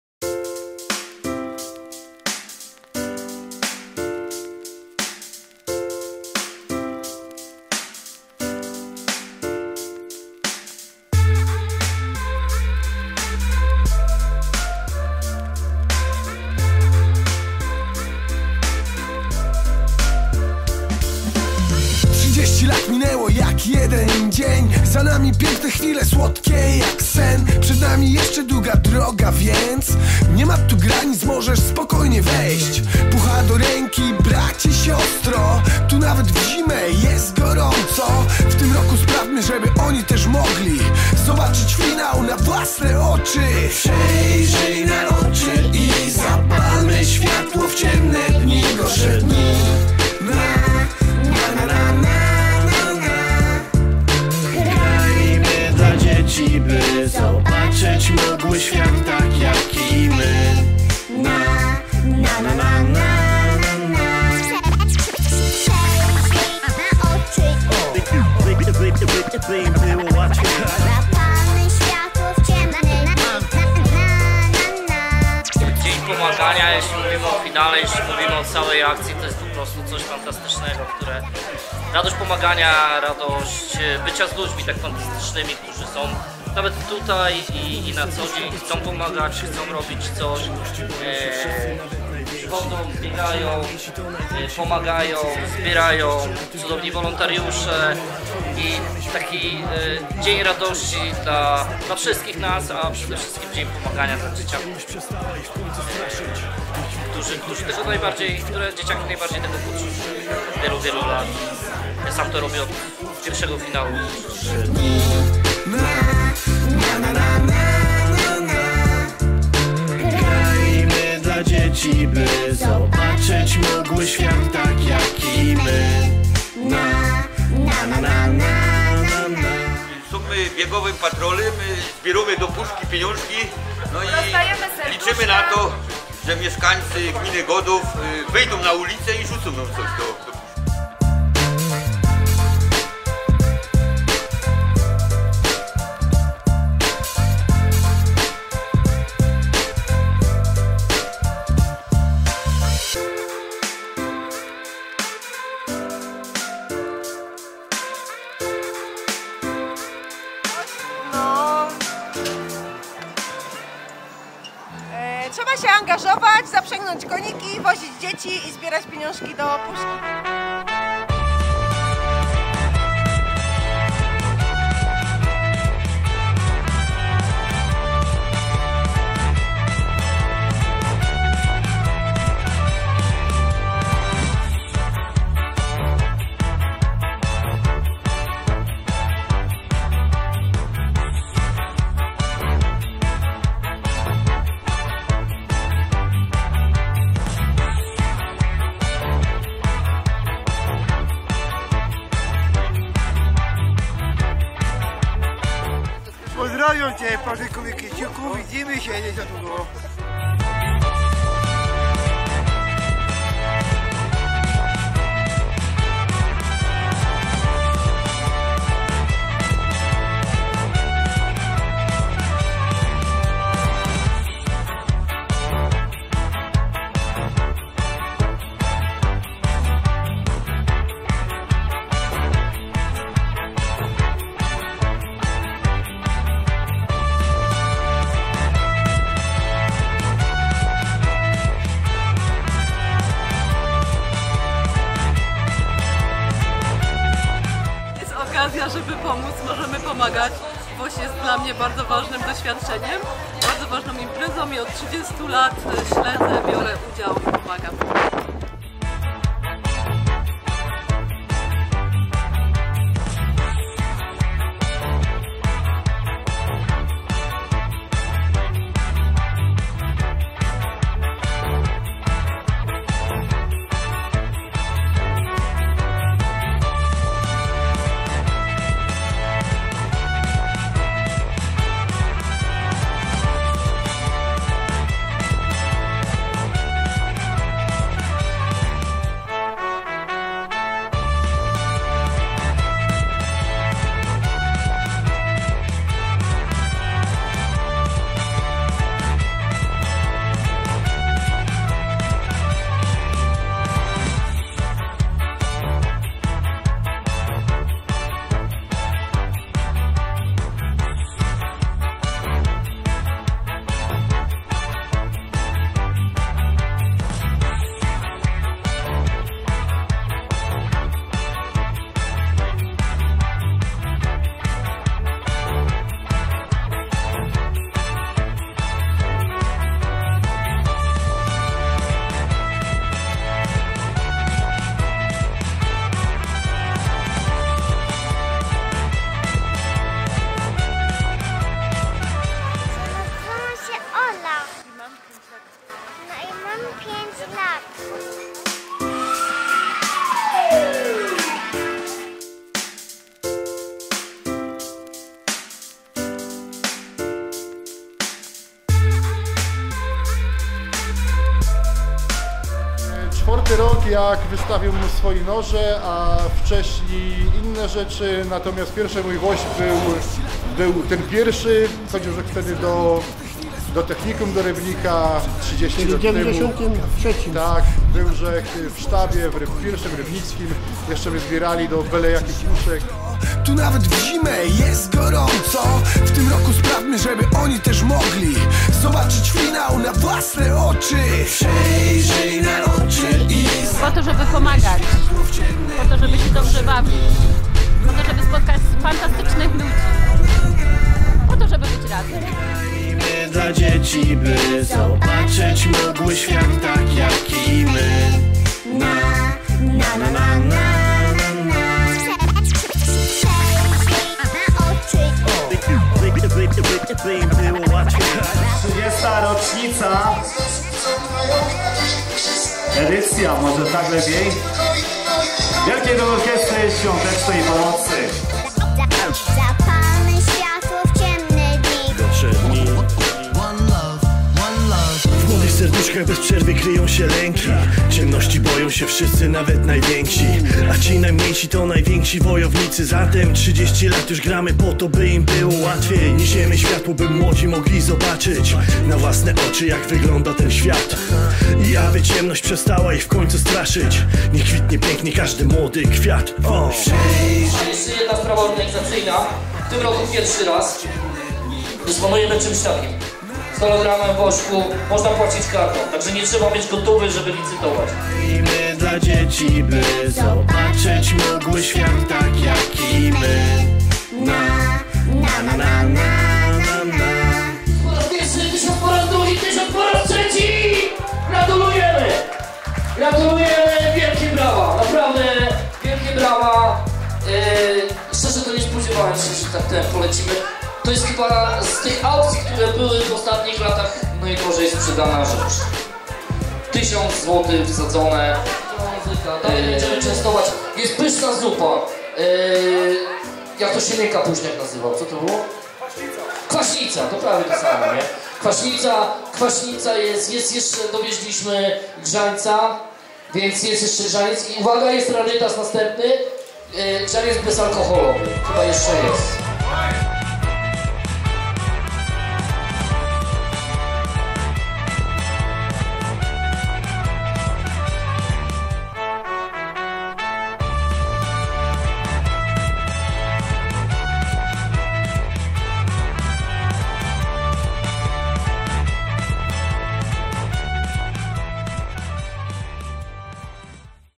WOŚP Godów 2022
WOŚP Godów 2022 - tak było w wielkim skrócie w Gminie Godów.